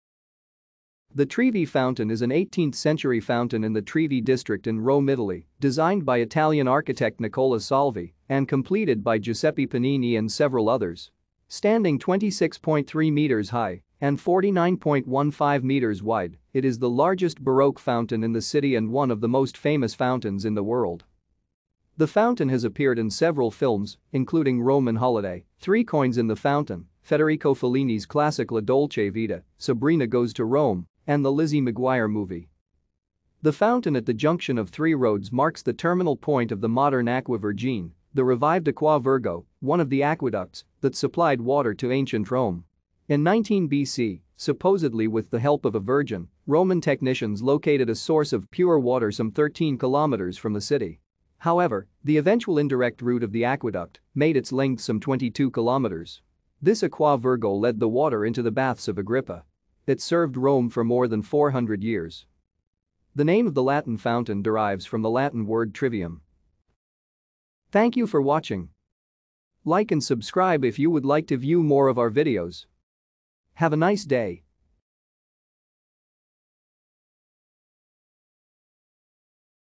Trevi Fountain
The Trevi Fountain  is an 18th-century fountain in the Trevi district in Rome, Italy, designed by Italian architect Nicola Salvi and completed by Giuseppe Pannini and several others. Standing 26.3 metres  high and 49.15 metres  wide, it is the largest Baroque fountain in the city and one of the most famous fountains in the world.